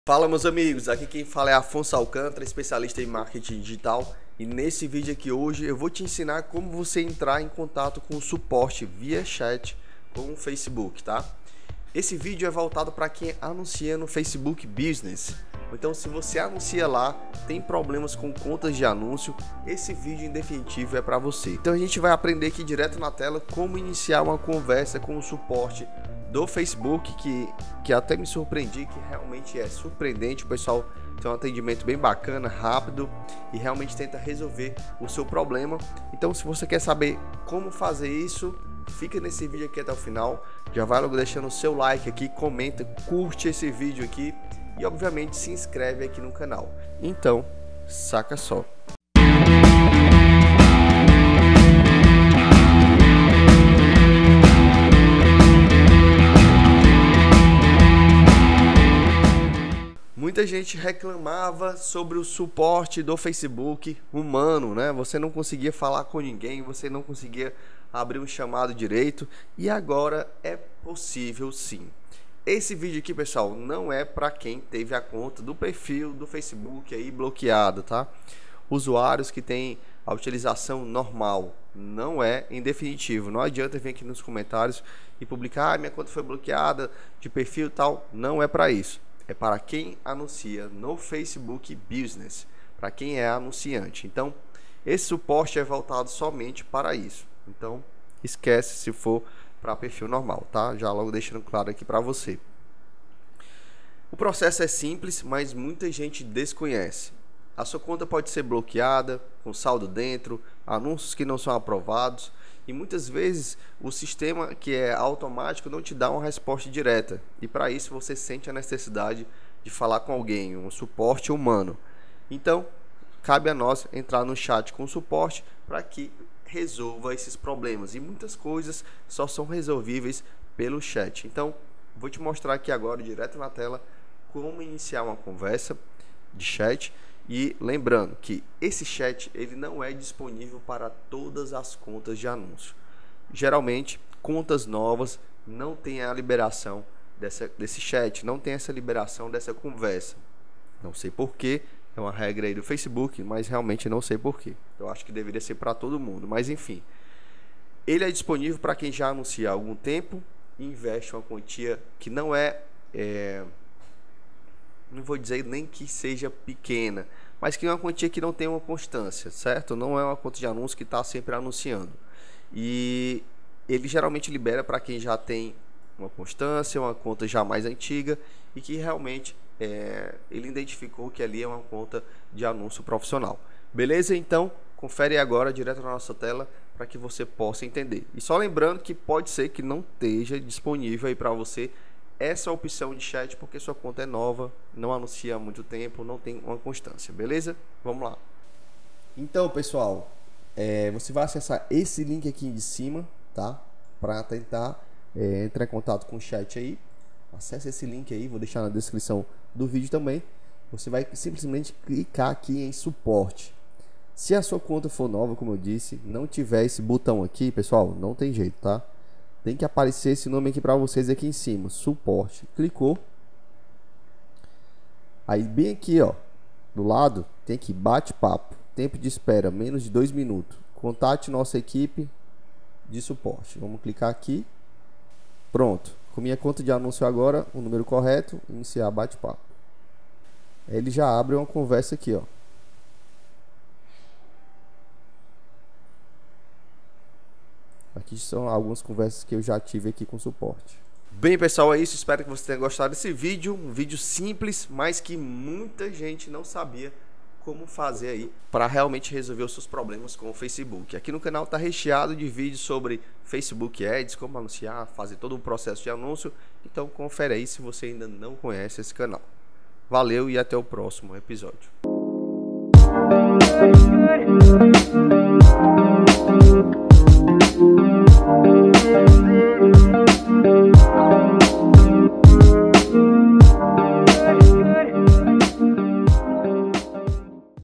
SUPORTE FACEBOOK ADS E INSTAGRAM VIA CHAT COMO FUNCIONA MARKETING DIGITAL
Tentar entrar em contato com o suporte do Facebook Ads e Instagram via chat para muitas pessoas é um pesadelo. Com esse vídeo vamos ajudar a você a fazer isso e facilitar seu marketing digital.

Acesse o link para iniciar o CHAT VIA SUPORTE DO FACEBOOK ADS

⚠ Precisa de Serviços de Marketing Digital?

⚠ Crie seu perfil no Vooice Rede Social criada por mim